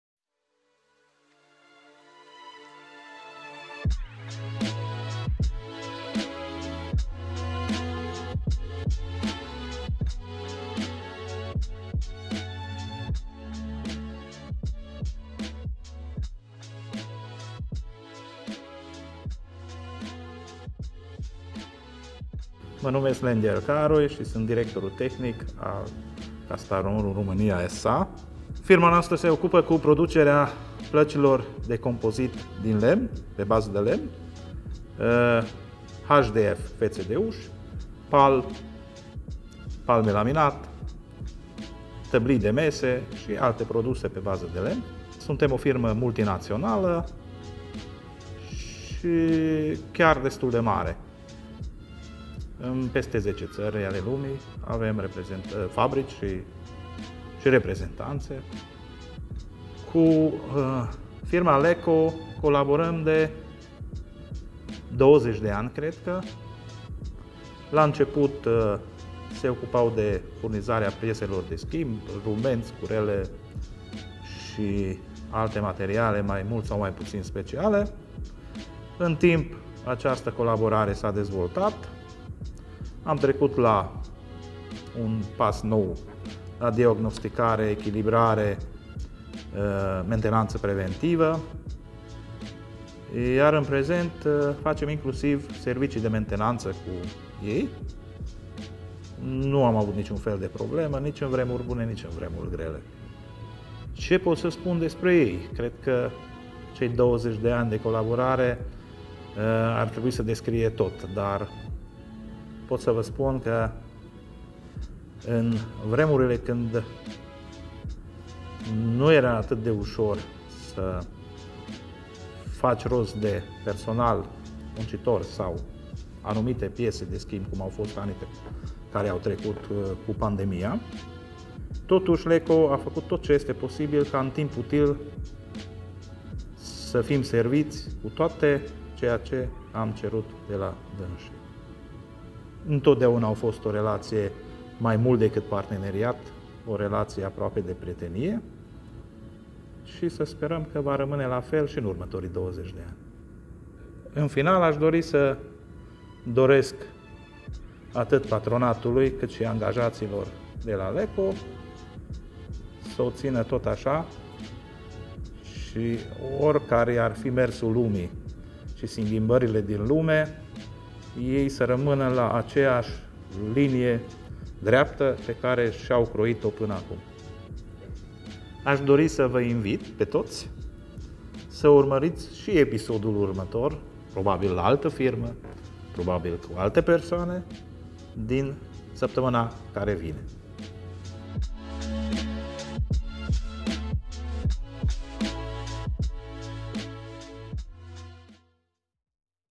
30 de ani de Leco, 20.000 de clienți și o echipă de milioane - Lengyel Károly, Kastamonu Romania SA
Te invităm să vizionezi următorul episod cu dl. Lengyel Károly - Director Tehnic SC Kastamonu Romania SA – unul dintre cei mai importanți clienți ai firmei Lecoimpex!
Rămâi aproape și urmărește povestea noastră și săptămâna viitoare!